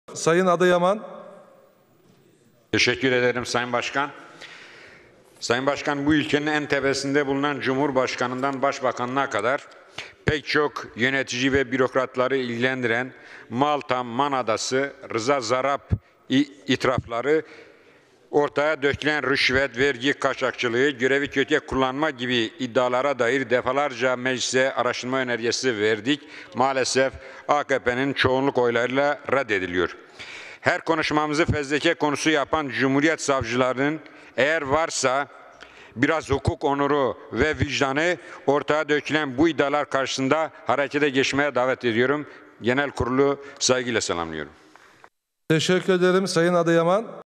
GK30 11 2017 M. Emin Adiyaman-HDP-IĞDIR : AKP'deki yolsuzluk sınırları aştı
HDP'li ADIYAMAN: Yolsuzluk 15 yıldır AKP iktidarı tarafından gelenek haline getirildi.